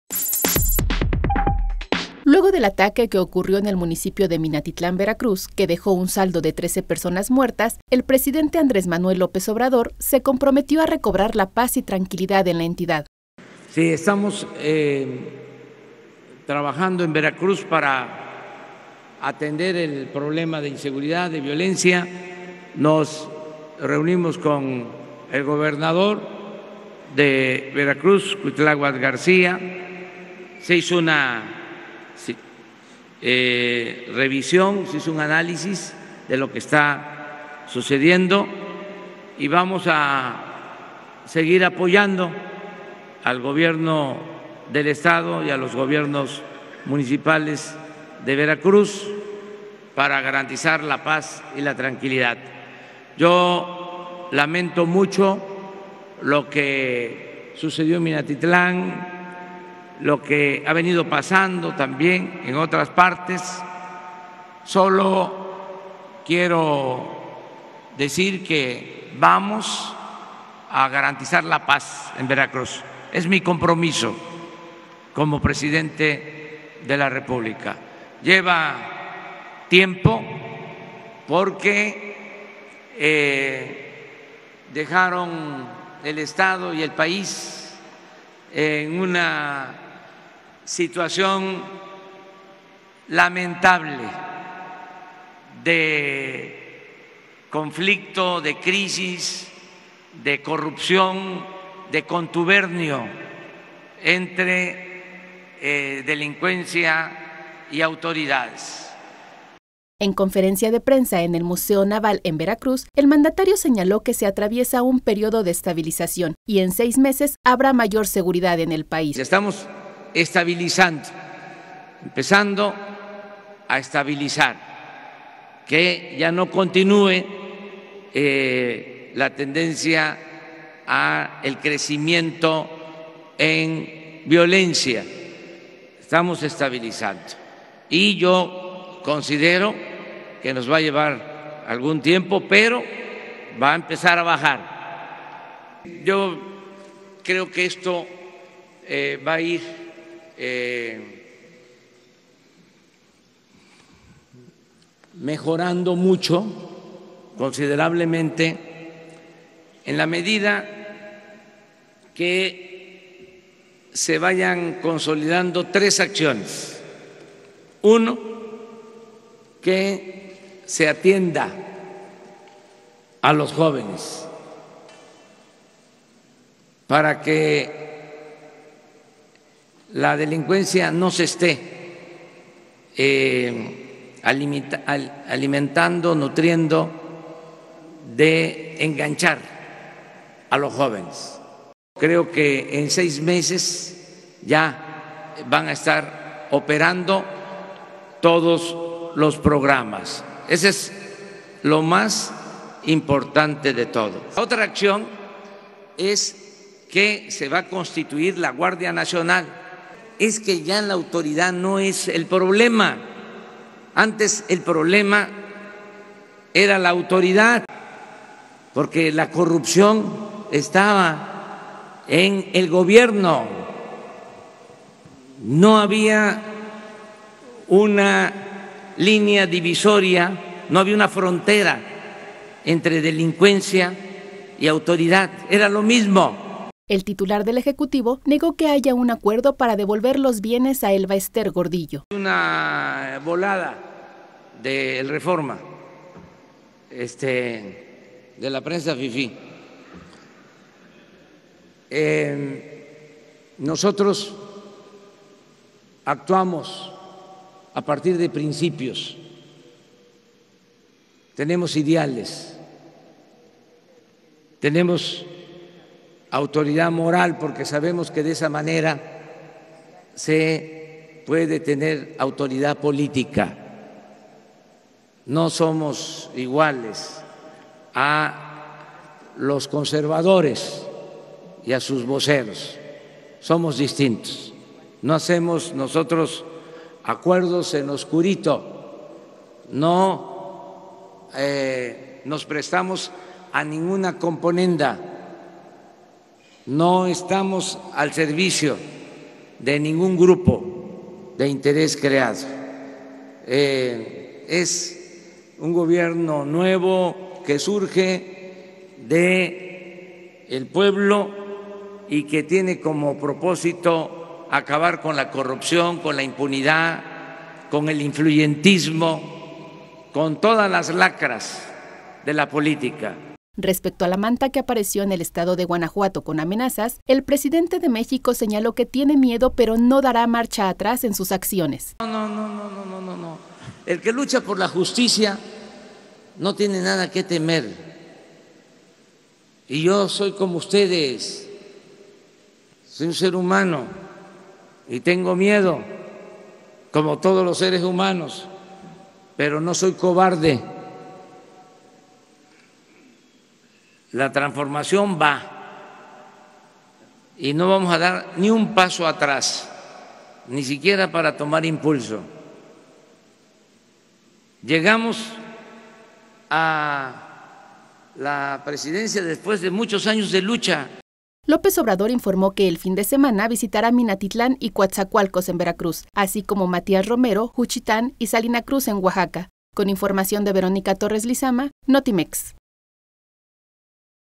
Garantizar seguridad, mi compromiso como presidente: AMLO
El titular del Ejecutivo negó que haya un acuerdo para devolver bienes a Elba Esther Gordillo.